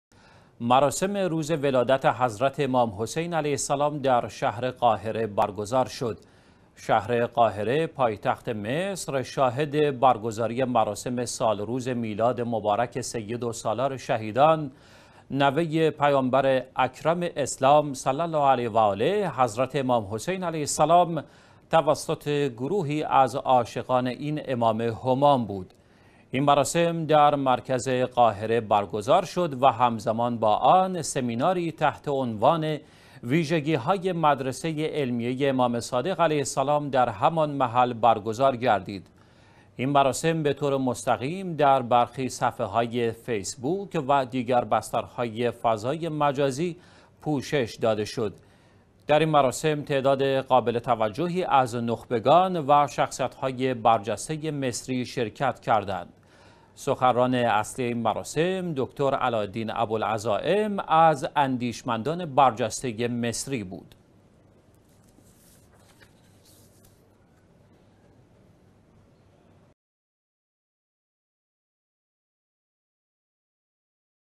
برگزاری مراسم ولادت امام حسین علیه السلام در مصر
مراسم روز ولادت حضرت امام حسین علیه السلام در شهر قاهره برگزار شد.
شهر قاهره پایتخت مصر شاهد برگزاری مراسم سالروز میلاد مبارک سید و سالار شهیدان، نوه پیامبر اکرم اسلام صلی الله علیه و آله، حضرت امام حسین علیه السلام توسط گروهی از عاشقان این امام همام بود.
این مراسم در مرکز قاهره برگزار شد و همزمان با آن، سمیناری تحت عنوان "ویژگی های مدرسه علمیه امام صادق علیه السلام" در همان محل برگزار گردید.
این مراسم به طور مستقیم در برخی صفحه های فیس بوک و دیگر بسترهای فضای مجازی پوشش داده شد.
در این مراسم تعداد قابل توجهی از نخبگان و شخصیت های برجسته مصری شرکت کردند.
سخنران اصلی این مراسم دکتر "علاء الدین أبوالعزائم" از اندیشمندان برجسته مصری بود.